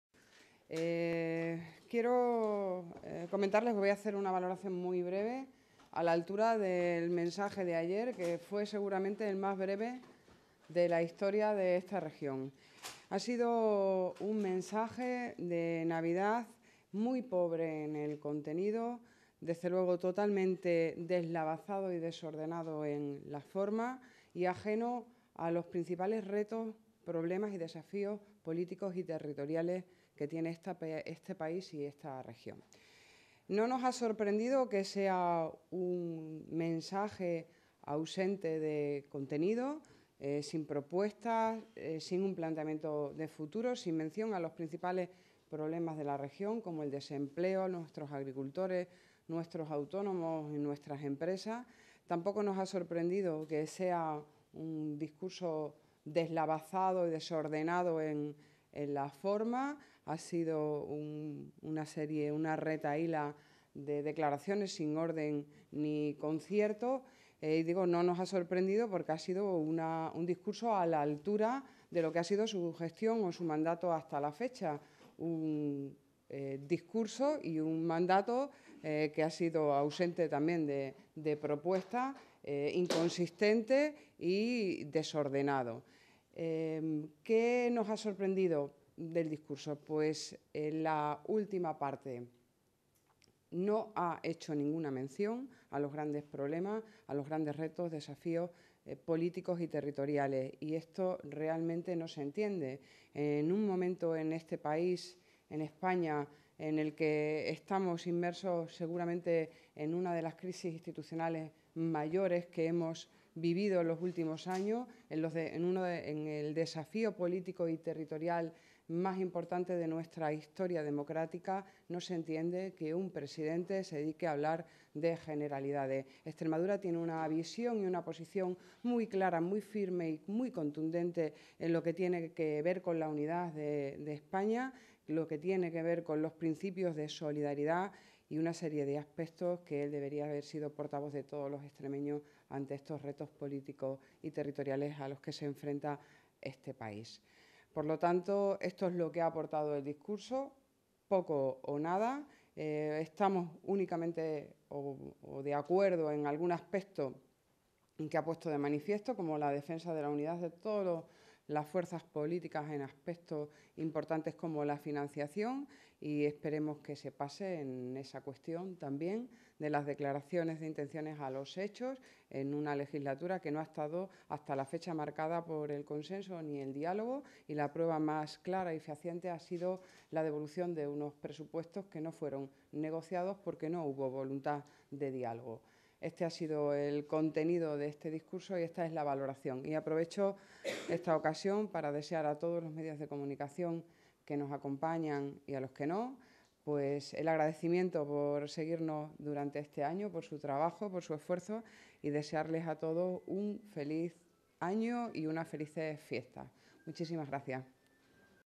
El PP extremeño critica el mensaje de Vara
La portavoz del Grupo Popular en la Asamblea de Extremadura, Cristina Teniente, ha considerado que el mensaje de Fin de Año pronunciado este pasado miércoles por el presidente de la Junta, Guillermo Fernández Vara, fue "pobre en el contenido, desordenado en las formas y ajeno a los problemas de la región".